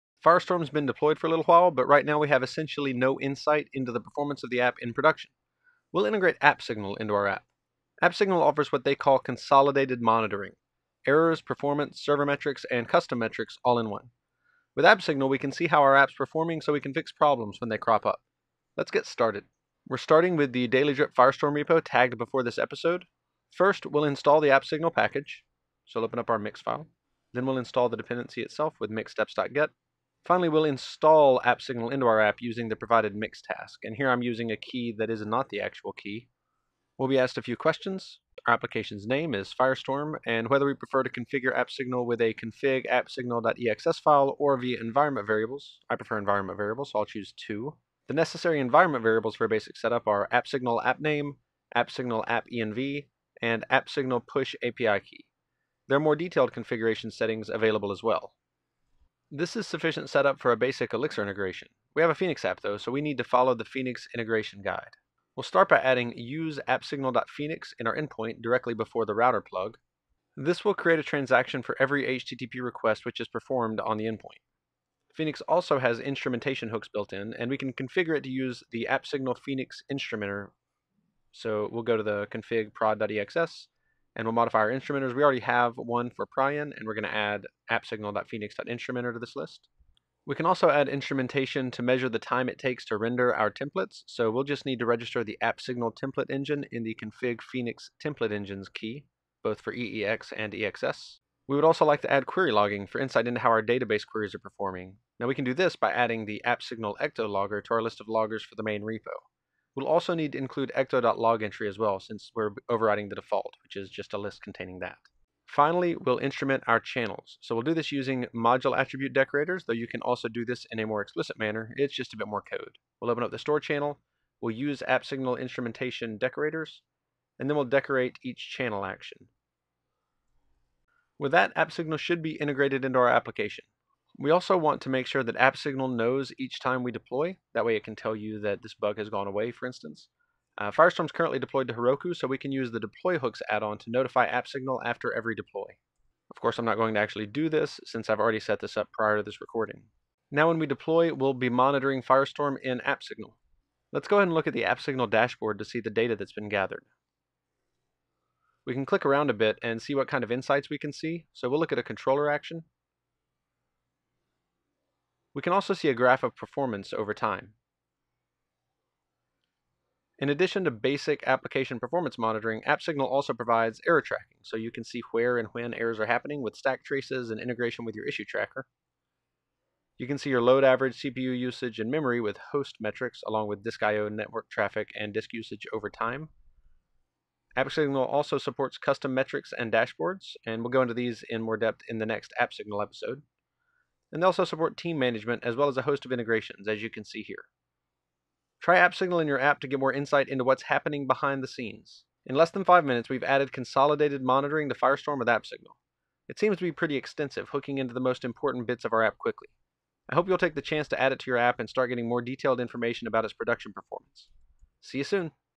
Monitoring with AppSignal
Add Performance Monitoring to a Phoenix app in five minutes or less.